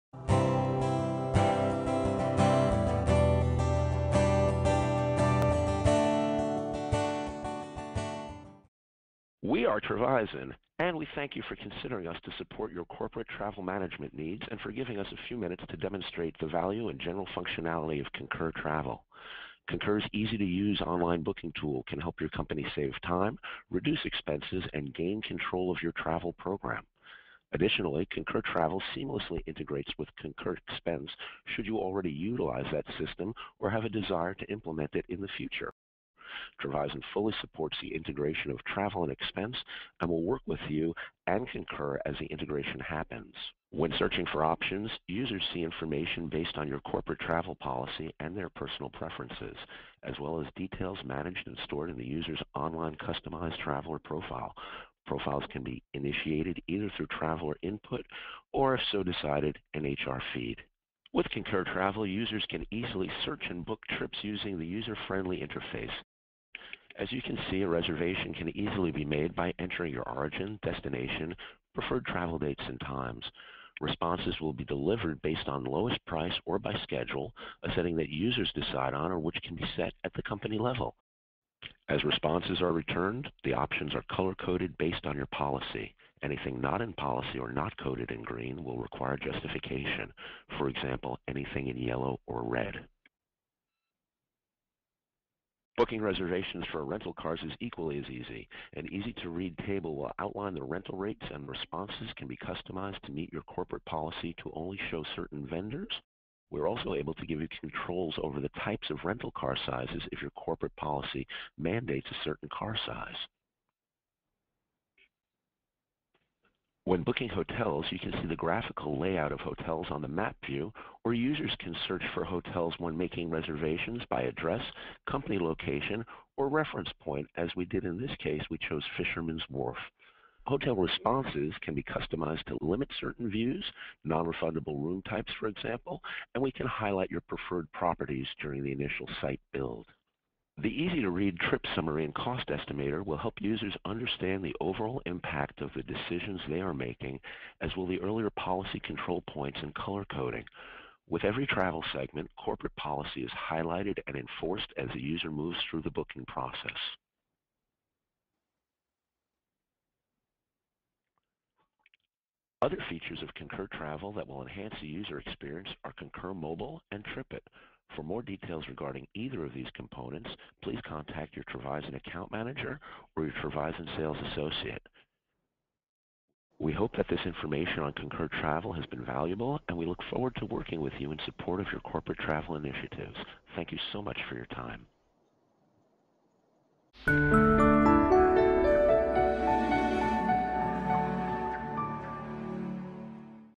Concur - Short Demo
Travizon is a corporate travel management company.  Please watch our short demonstration of the Concur Travel online booking tool.